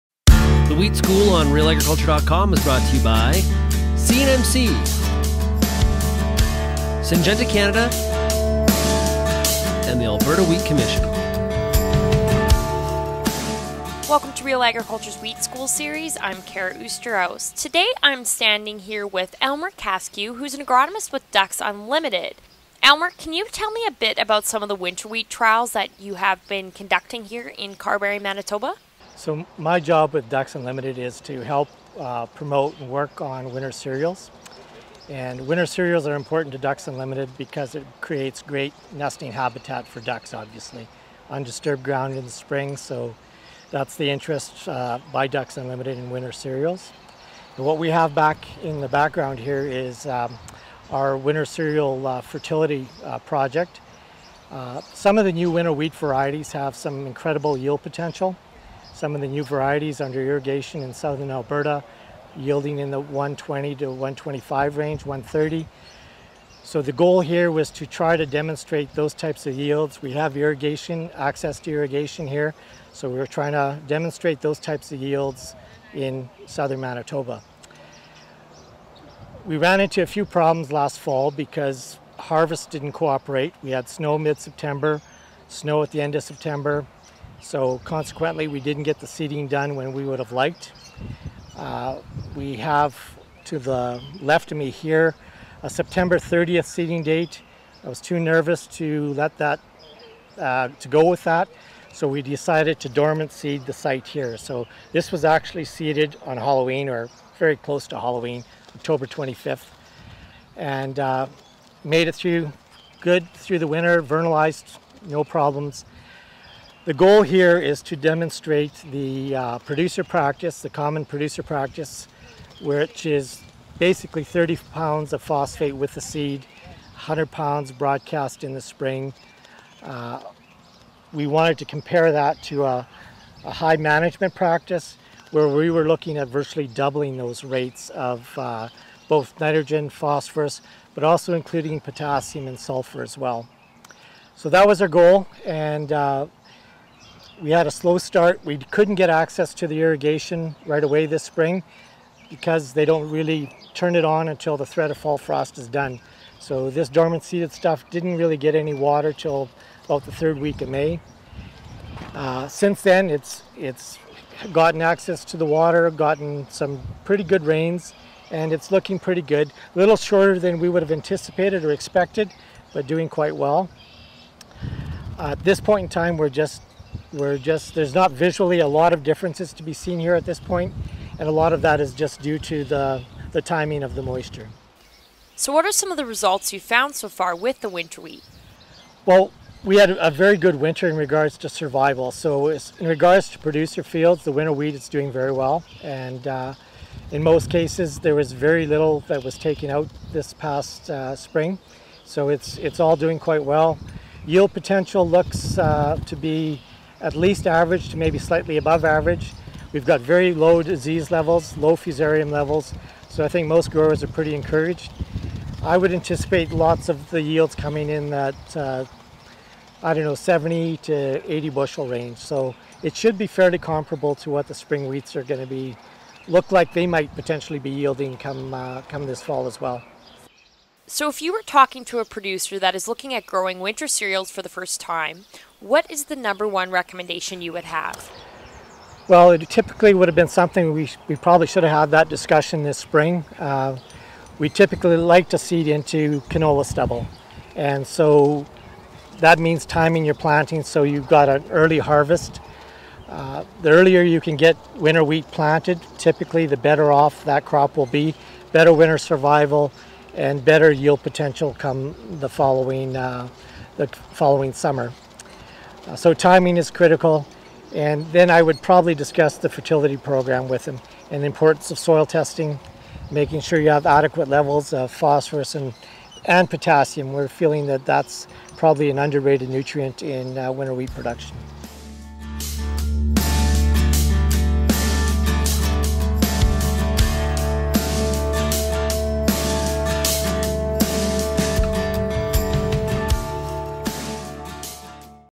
Wheat School: Feeding winter wheat
It's time for farmers to be thinking about seeding winter wheat, but many questions still remain around the crop such as: when do I seed it? What sort of yields can I expect? How frost resistant is the variety? Always top of mind for this crop, too, is fertility — how much is enough and when should I apply fertilizer?

RealAgriculture's Kara Oosterhuis caught up with Elmer Kaskiw, agronomist with Ducks Unlimited Canada, at Crops-A-Palooza at Carberry, Man., to talk about some of the fertility trials that have been conducted in the southern part of the province.